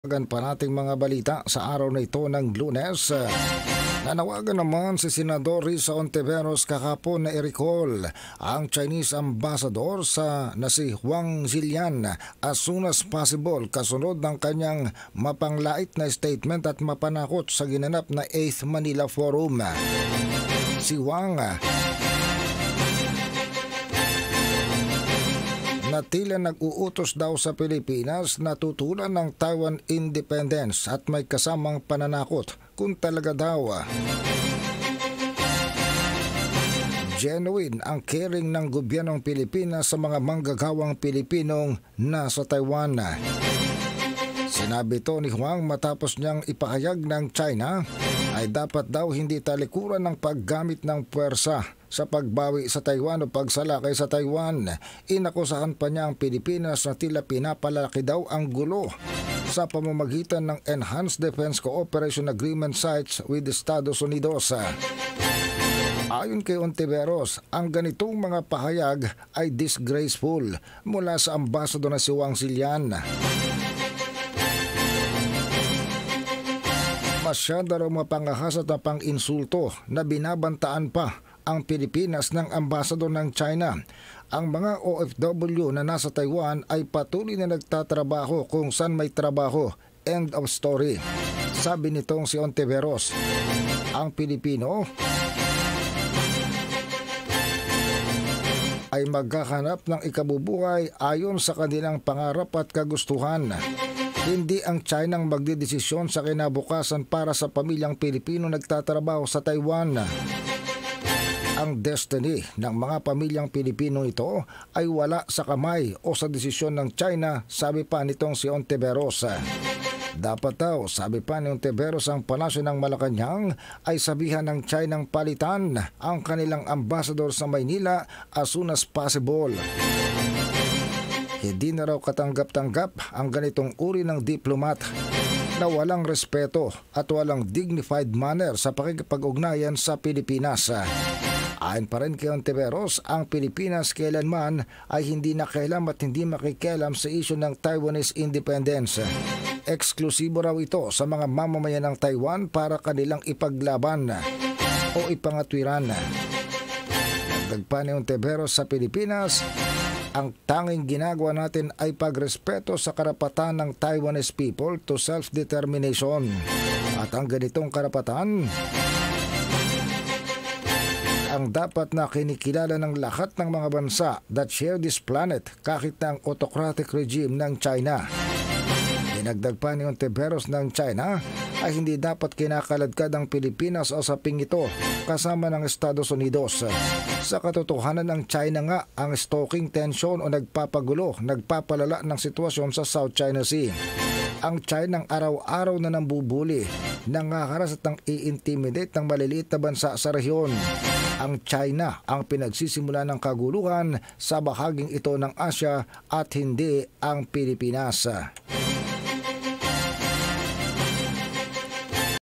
𝐍𝐚𝐧𝐚𝐰𝐚𝐠𝐚𝐧 𝐬𝐢 𝐒𝐞𝐧. 𝐇𝐨𝐧𝐭𝐢𝐯𝐞𝐫𝐨𝐬 𝐧𝐚 𝐢-𝐫𝐞𝐜𝐚𝐥𝐥 𝐚𝐧𝐠 𝐂𝐡𝐢𝐧𝐞𝐬𝐞 𝐚𝐦𝐛𝐚𝐬𝐬𝐚𝐝𝐨𝐫 𝐦𝐚𝐭𝐚𝐩𝐨𝐬 "𝐝𝐢𝐬𝐠𝐫𝐚𝐜𝐞𝐟𝐮𝐥 𝐬𝐭𝐚𝐭𝐞𝐦𝐞𝐧𝐭𝐬"
𝐍𝐚𝐧𝐚𝐰𝐚𝐠𝐚𝐧 𝐬𝐢 𝐒𝐞𝐧. 𝐑𝐢𝐬𝐚 𝐇𝐨𝐧𝐭𝐢𝐯𝐞𝐫𝐨𝐬 𝐧𝐚 𝐝𝐚𝐩𝐚𝐭 𝐡𝐢𝐦𝐮𝐤𝐢𝐧 𝐧𝐠 𝐩𝐚𝐥𝐚𝐬𝐲𝐨 𝐚𝐧𝐠 𝐁𝐞𝐢𝐣𝐢𝐧𝐠 𝐧𝐚 𝐢-𝐫𝐞𝐜𝐚𝐥𝐥 𝐚𝐧𝐠 𝐂𝐡𝐢𝐧𝐞𝐬𝐞 𝐚𝐦𝐛𝐚𝐬𝐬𝐚𝐝𝐨𝐫 𝐭𝐨 𝐭𝐡𝐞 𝐏𝐡𝐢𝐥𝐢𝐩𝐩𝐢𝐧𝐞𝐬 𝐦𝐚𝐭𝐚𝐩𝐨𝐬 𝐦𝐚𝐠𝐩𝐚𝐡𝐚𝐲𝐚𝐠 𝐧𝐠 𝐦𝐚𝐢𝐭𝐮𝐭𝐮𝐫𝐢𝐧𝐠 𝐧𝐚 "𝐝𝐢𝐬𝐠𝐫𝐚𝐜𝐞𝐟𝐮𝐥" 𝐚𝐭 𝐡𝐢𝐧𝐝𝐢 𝐤𝐚𝐭𝐚𝐧𝐠𝐠𝐚𝐩-𝐭𝐚𝐧𝐠𝐠𝐚𝐩, 𝐤𝐚𝐦𝐚𝐤𝐚𝐫𝐚𝐚𝐧 𝐬𝐚 𝟖𝐭𝐡 𝐌𝐚𝐧𝐢𝐥𝐚 𝐅𝐨𝐫𝐮𝐦